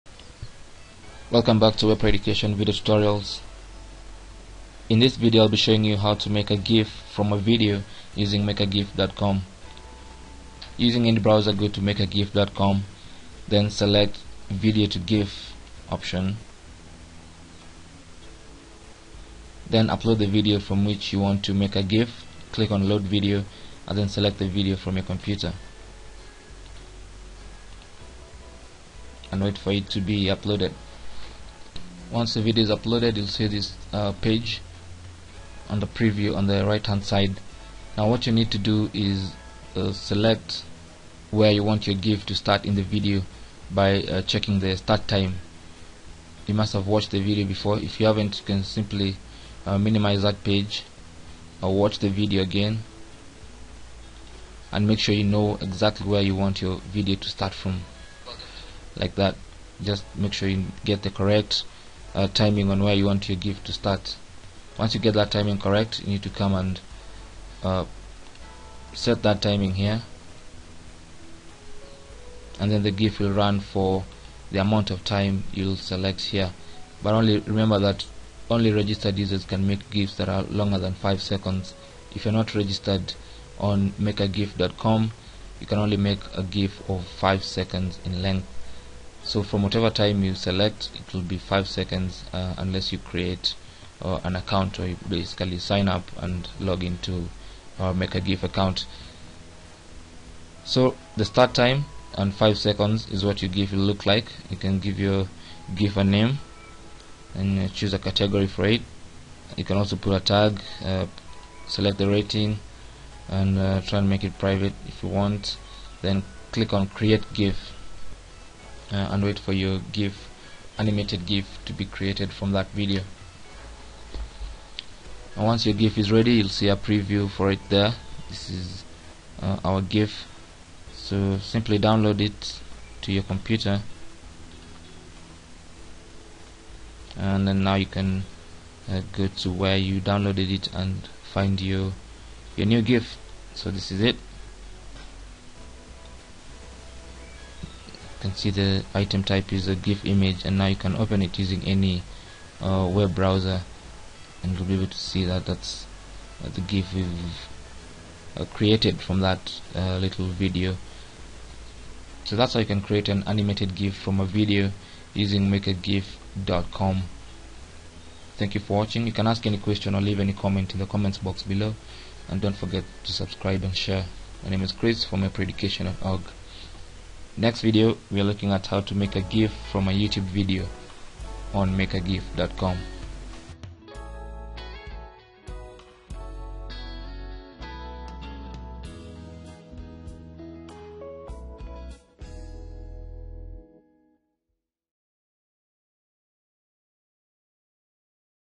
How To Make a Gif From a Video
Learn how to make a simple animated gif from a video saved on your computer.  In this tutorial,  we are using an online tool: makeagif.com